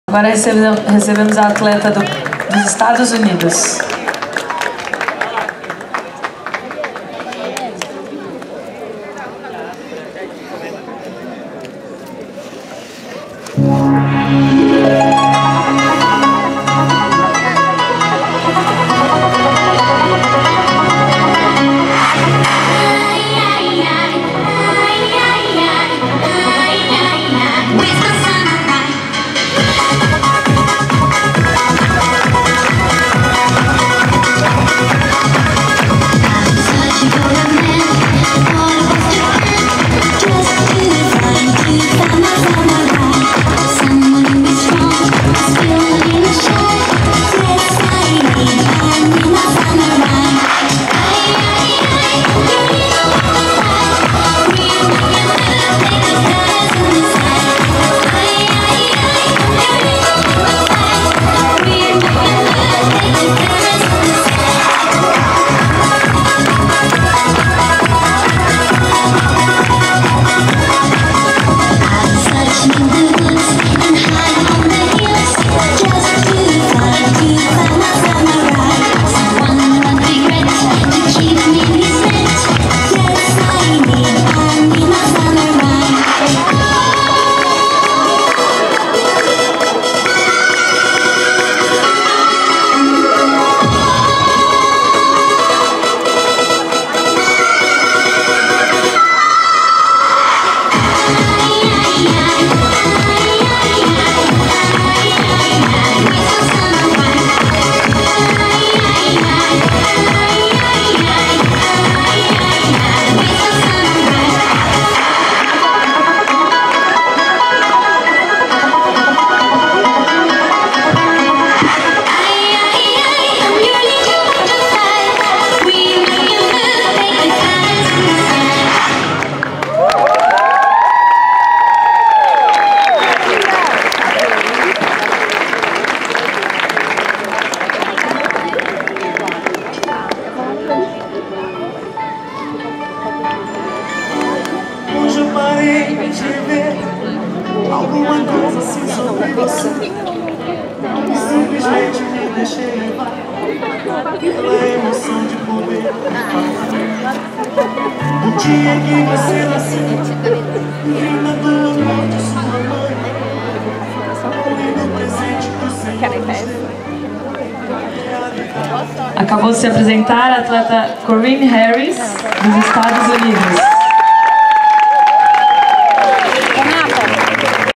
Corinne Swims Synchro Routine in Brazil
Corinne is an athlete with disabilities, living with autism. She hopes to inspire other athletes with disabilities around the world.

This synchro solo performance was during the 2nd International Symposium & Exhibition on Synchronized Swimming for Athletes with Disabilities hosted by Inspara in Brazil, September 2016. The global movement holds the aspiration to include synchronized swimming as a future Paralympic sport.

Cypress Swim Club, Monterey County, CA~USA